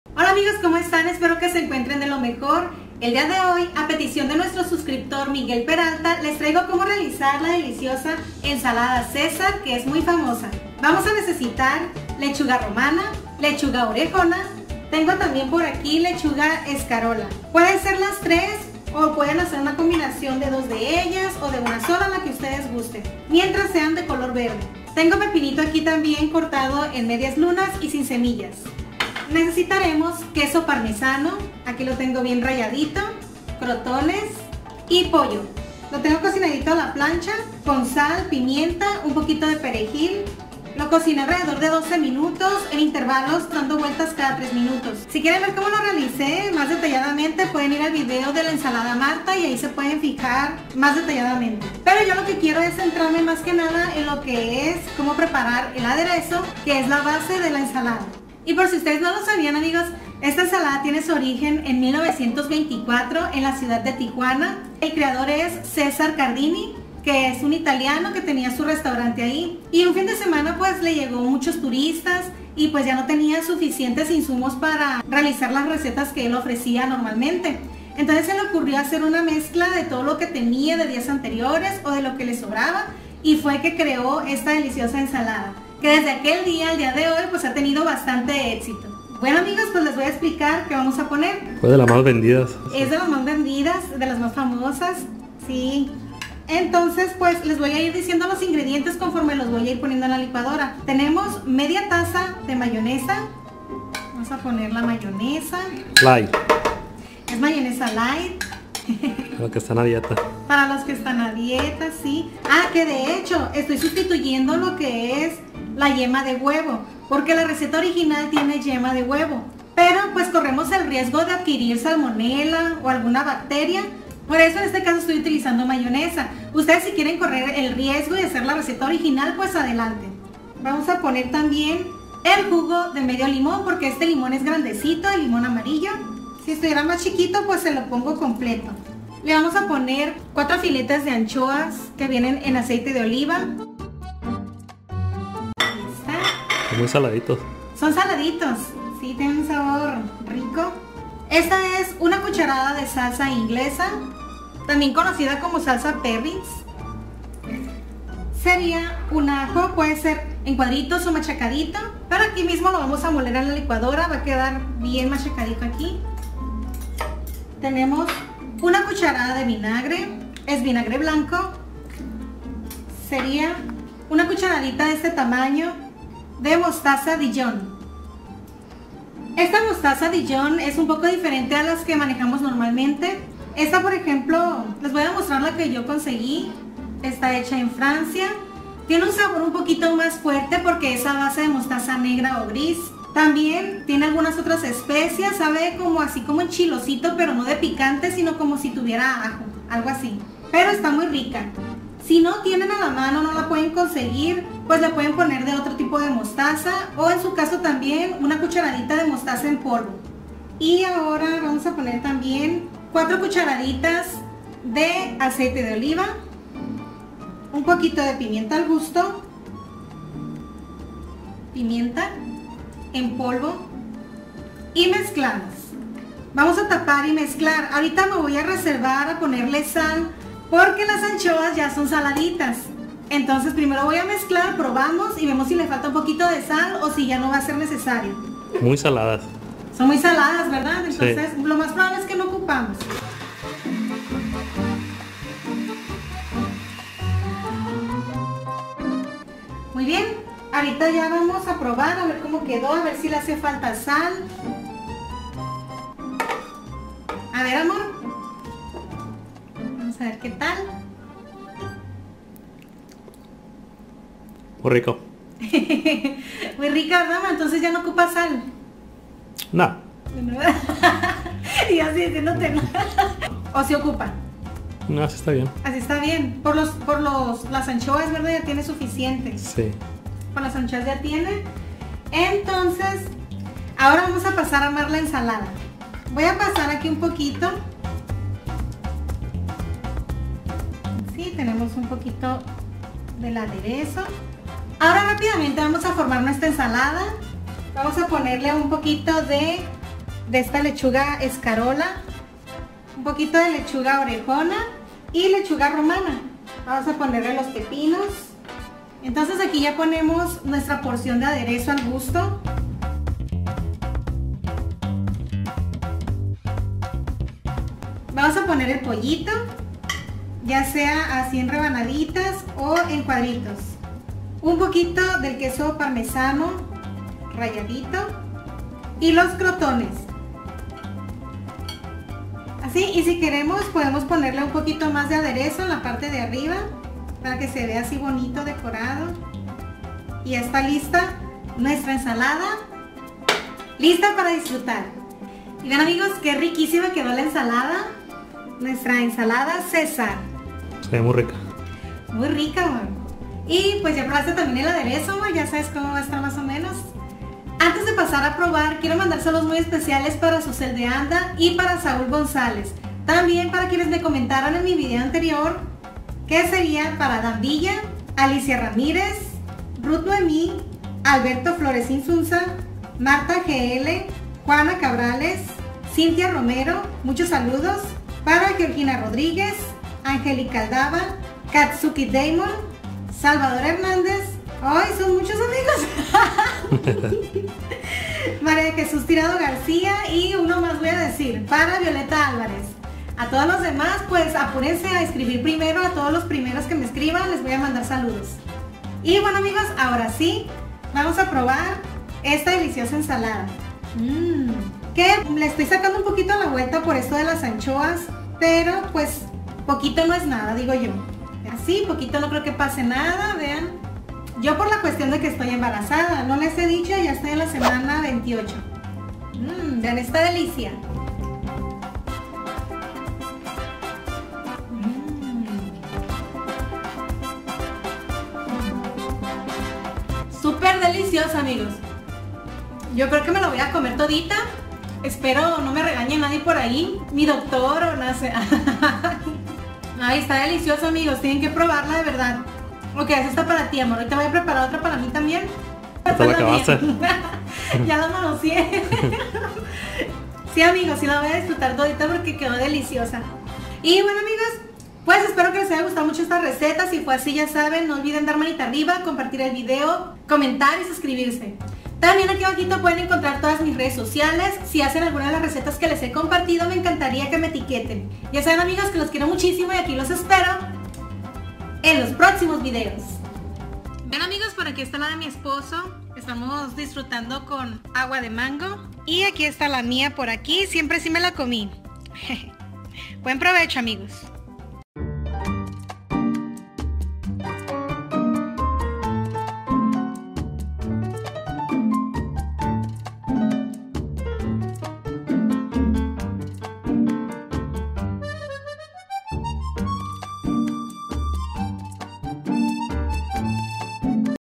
COMO HACER ENSALADA CESAR
Hoy les traigo como preparar la famosa ensalada cesar, esta famosa ensalada fue creada en el año de 1924 en un restaurante aquí en Mexico en la ciudad de Tijuana por cesar cardini de origen italiano, en un fin de semana recibió una multitud de turistas y no tenia los ingredientes suficientes  por lo que tomo de los ingredientes que encontró y realizo esta mezcla de la nada por lo que se dice que fue de pura casualidad pero sus comensales quedaron tan encantados, por lo que esta receta a sido un gran exito desde ese dia hasta el dia de hoy.

Ingredientes :
1/4 de lechuga romana
1/4 de lechuga orejona
1/4 de lechuga escarola
1 pepino
100 gr queso parmesano
 crotones

para el aderezo:
4  filetes  Anchoas
1 cucharadita  Mostaza dijon
1 diente ajo picadito o machacados
 2 cdas  Aceite de oliva
½ taza de mayonesa (o  Huevos crudos las yemas )
3 cucharadas soperas de queso parmesano
1 cda de Salsa inglesa , perrins o worcestershire
1 cda de vinagre
El Jugo de un limón

PREPARA TAMBIEN ESTAS DELICIOSAS RECETAS♥♥♥